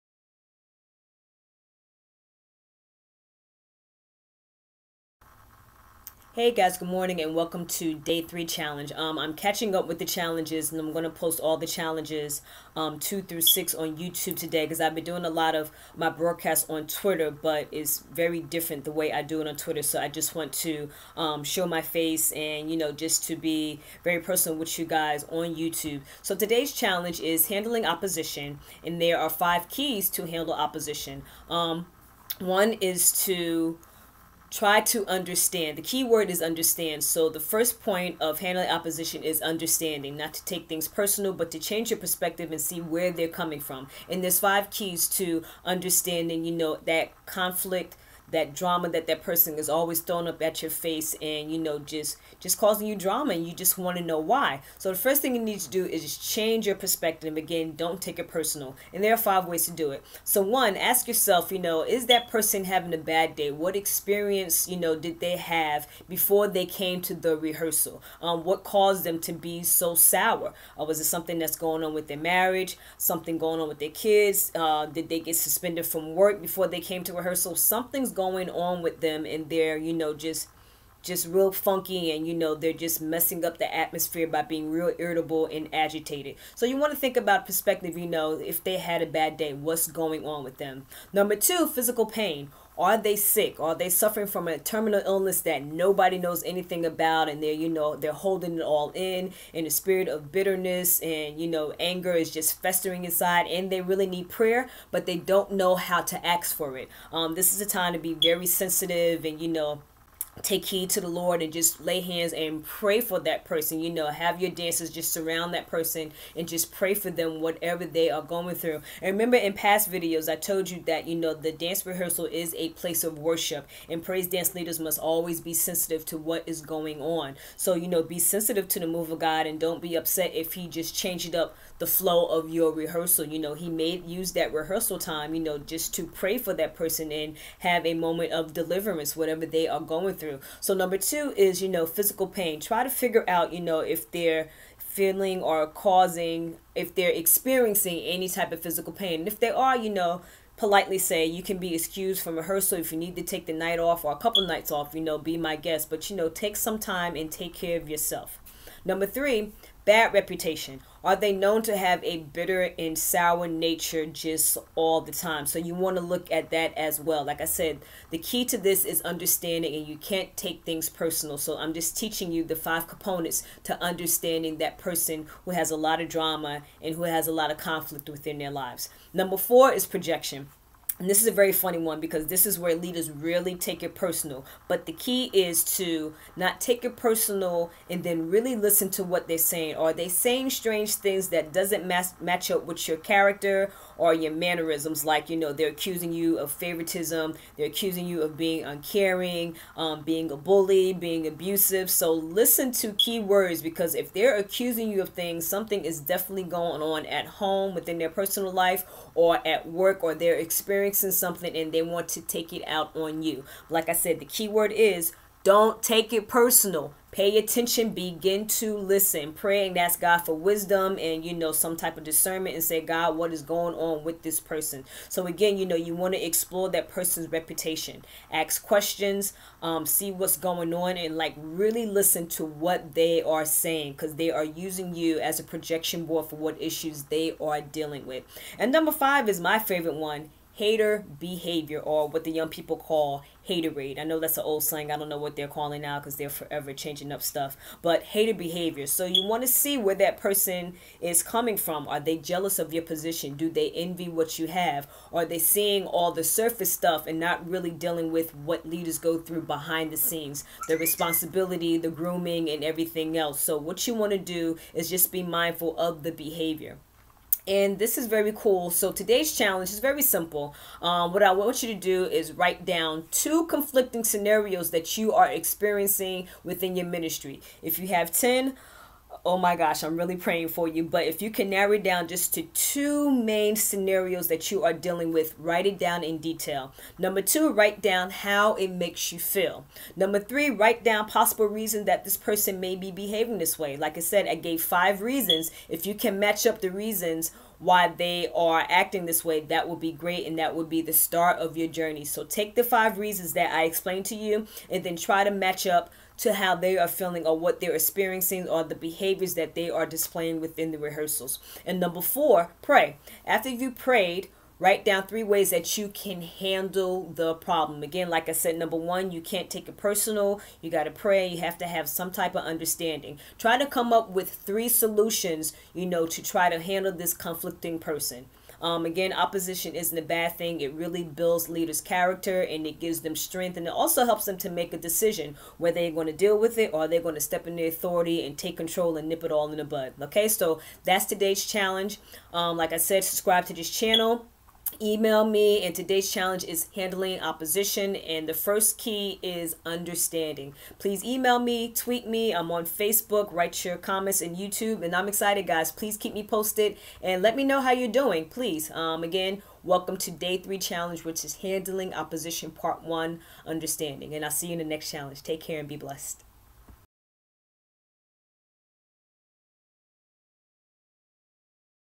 TOB 30 Day Praise Dance Leader Day 3 Challenge: Handling Oppostion
Welcome to Day 3 Challenge: Handling Opposition (Pt 1) Learn How to implement the 1st step and take the challenge. Today's topic is:UNDERSTANDING!